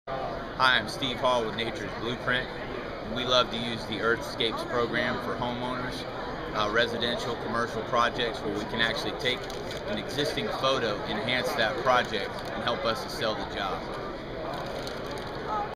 Nature's Blueprint - Selling Landscape Design Projects
- Steve with Nature's Blueprint in Alva, Florida shares how EARTHSCAPES sells more landscape design projects for residential and commercial clients.